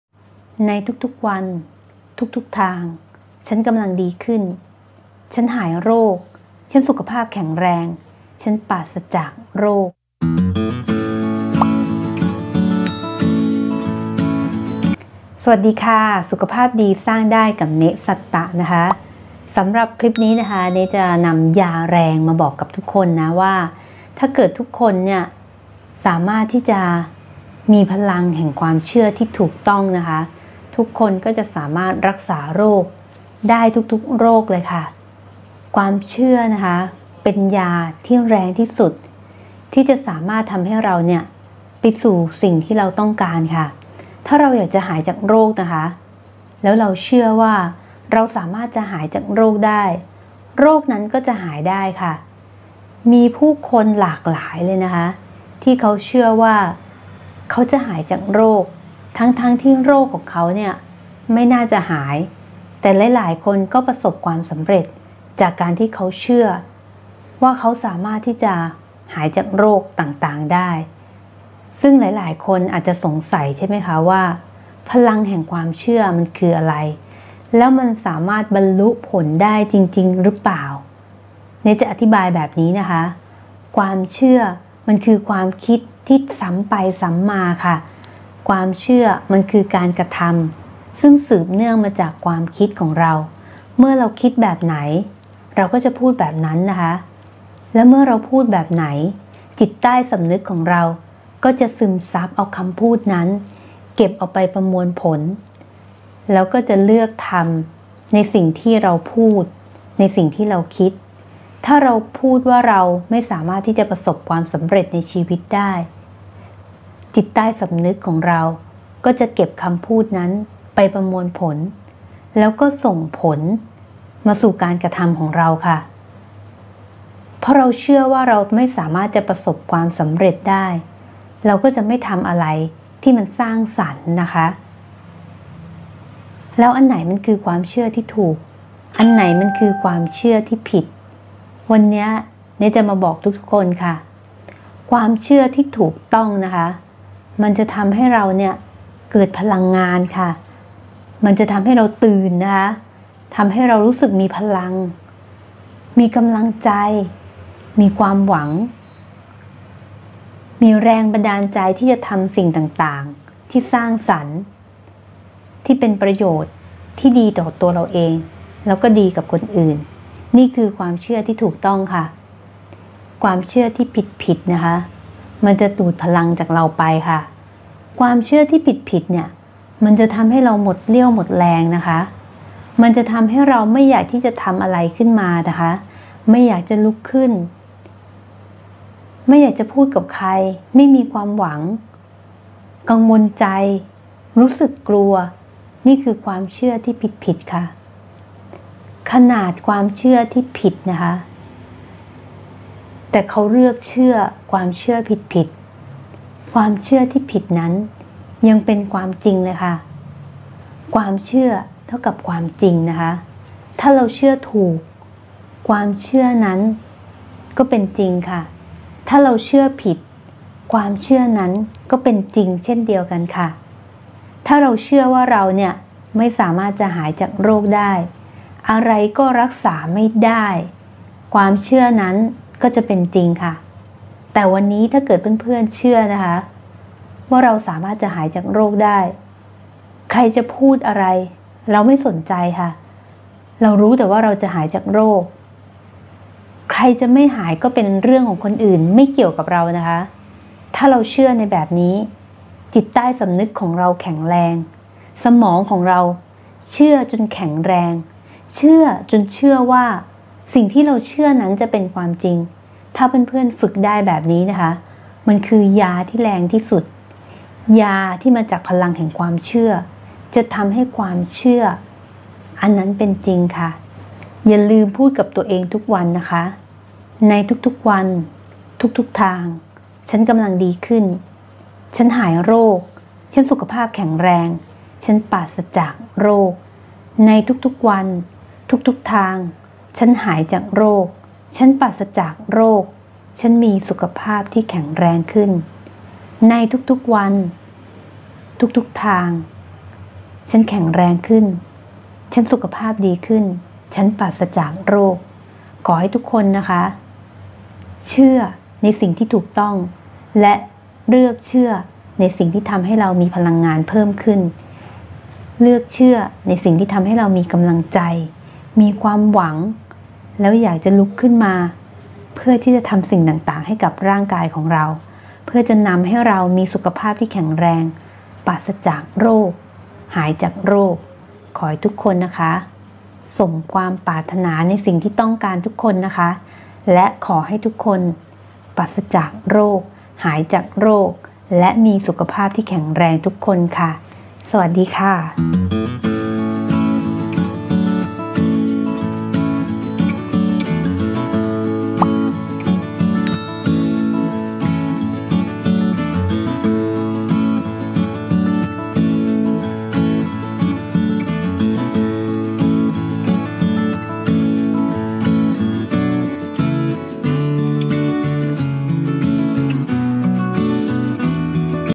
พลังแห่งความเชื่อรักษาโรคได้
ความเชื่อที่ถูกต้องจะทำให้เราหายจากโรคได้จริงๆค่ะ